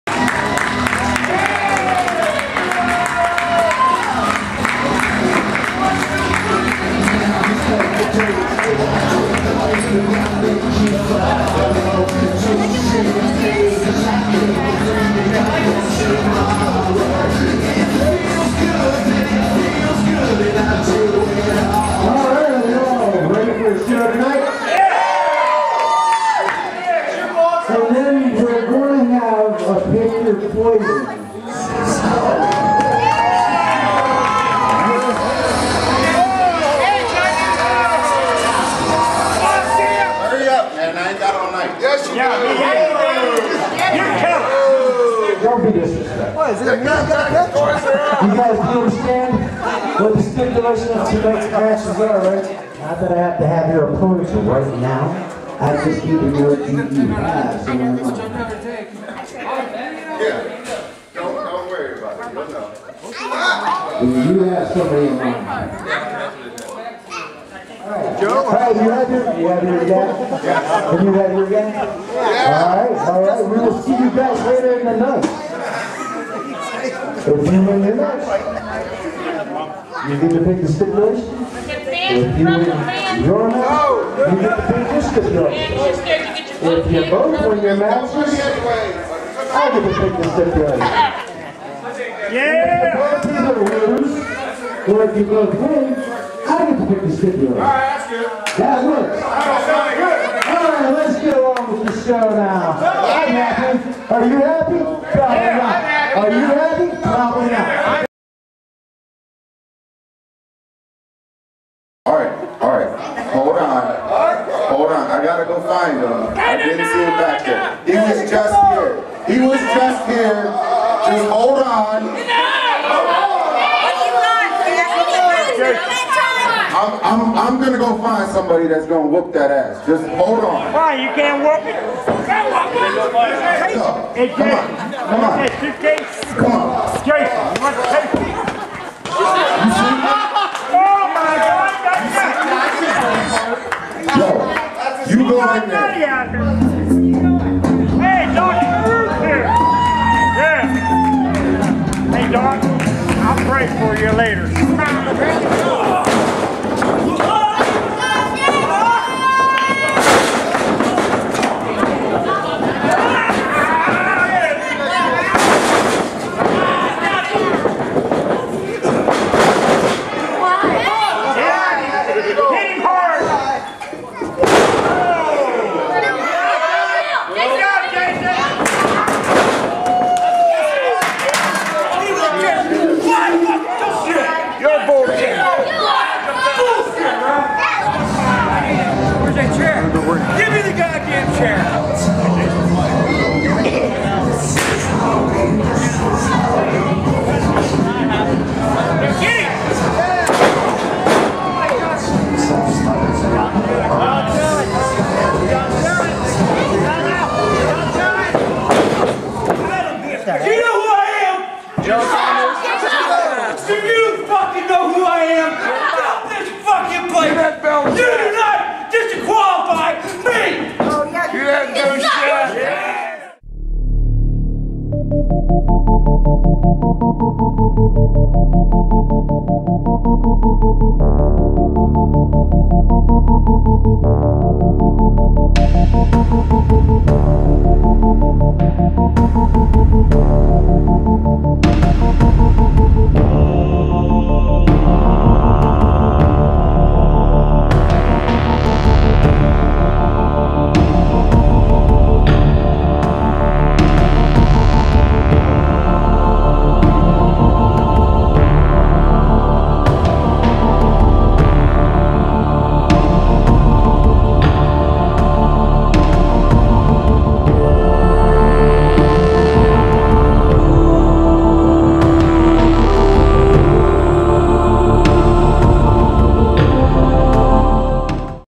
MEWL : Commissioner Lones Oaks or The Evil Dr. LeRoux?
At Main Event World League's event Betrayed, Commissioner Lones Oaks might have overstepped his boundaries...or did he? Watch the video and you decide!